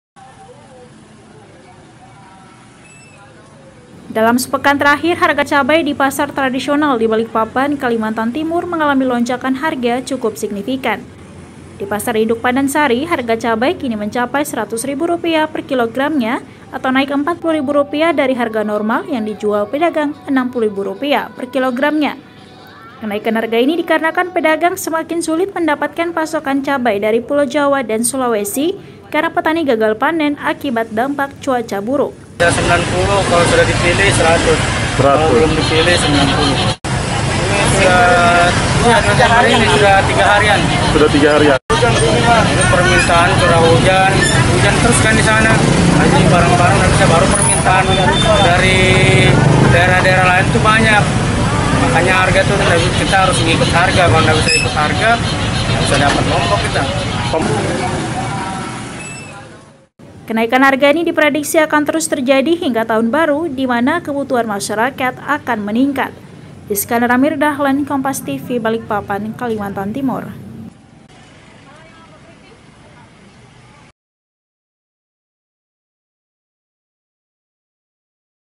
Pasokan Cabai dari Jawa dan Sulawesi ke Balikpapan Berkurang
BALIKPAPAN, KOMPAS.TV  Dalam sepekan terakhir harga cabai di pasar tradisional di Balikpapan, Kalimantan Timur mengalami lonjakan harga cukup signifikan.

Di pasar induk Pandan Sari, harga cabai kini mencapai Rp. 100 ribu per kilogramnya, atau naik Rp. 40 ribu dari harga normal yang dijual pedagang Rp. 60 ribu per kilogramnya.

Kenaikan harga ini dikarenakan pedagang semakin sulit mendapatkan pasokan cabai dari pulau Jawa dan Sulawesi, karena petani gagal panen akibat dampak cuaca buruk.

Kenaikan harga ini diprediksi akan terus terjadi hingga tahun baru, dimana kebutuhan masyarakat akan meningkat.